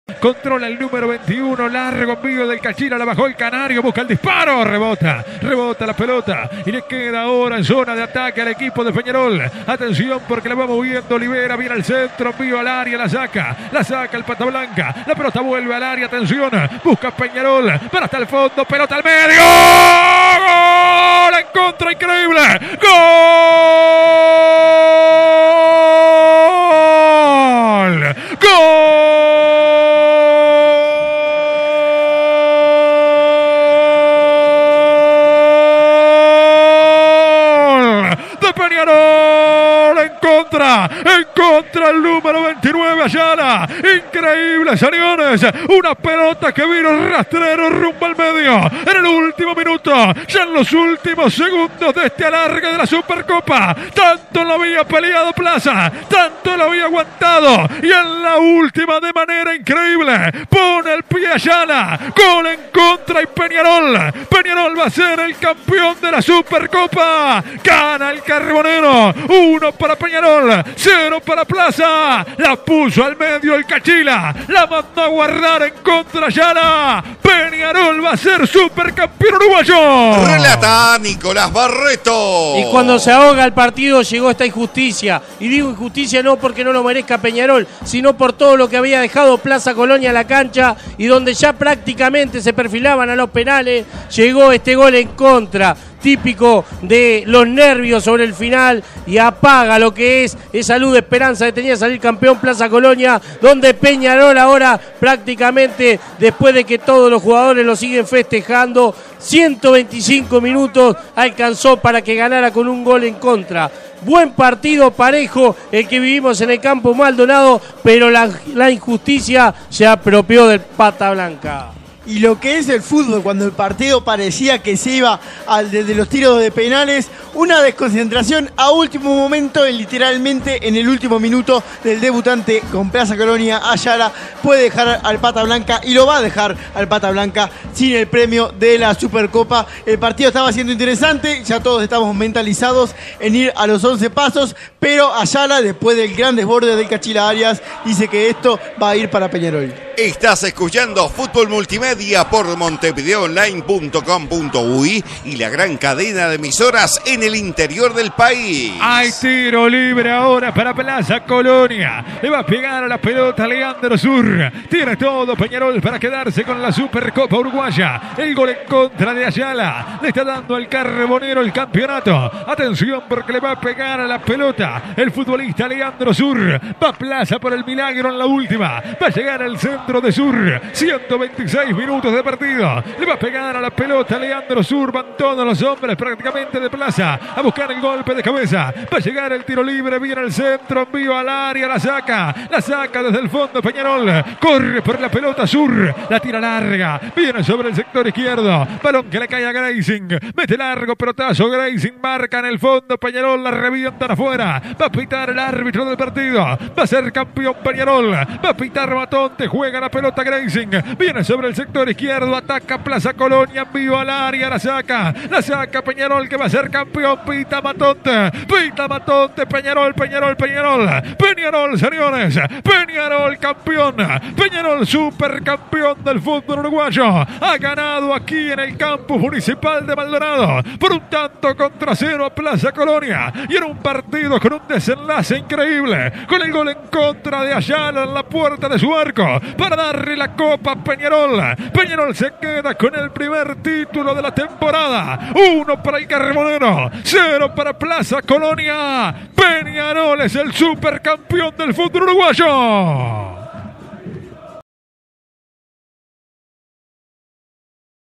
Peñarol Campeón Supercopa 2022 (por Nicolás Barreto)
El gol y el final del partido, por Nicolás Barreto y el equipo de Fútbol Multimedia para Montevideo Online.